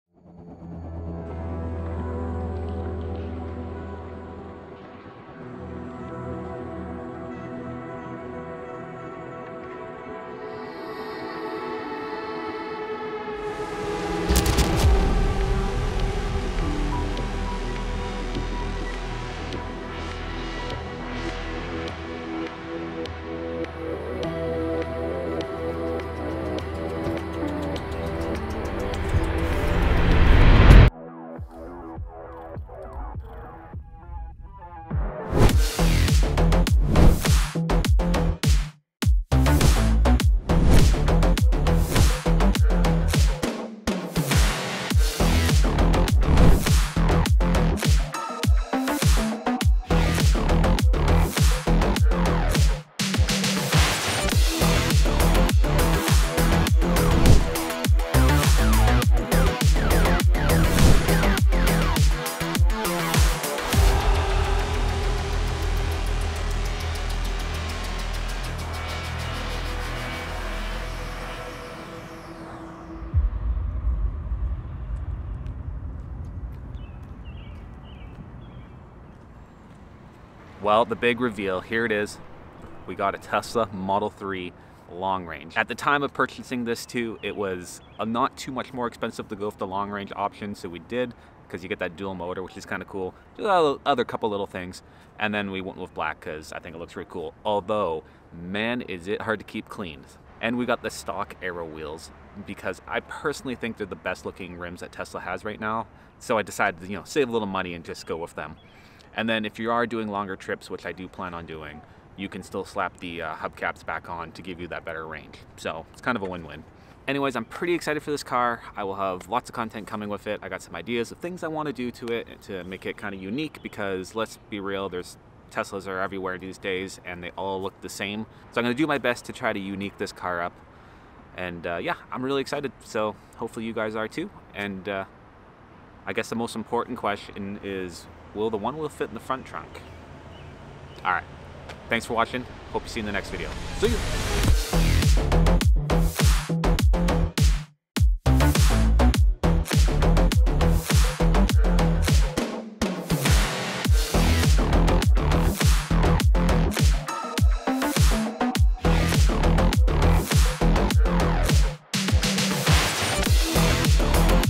2022 Tesla Model 3 Long Range // Our New Car!
Our New Car! that im VERY stoked on! Let me know if there is anything you want to see or know about this car!

Second Camera - Sony A6400
Main Lens -  Tamron 17-28mm F2.8

Thanks!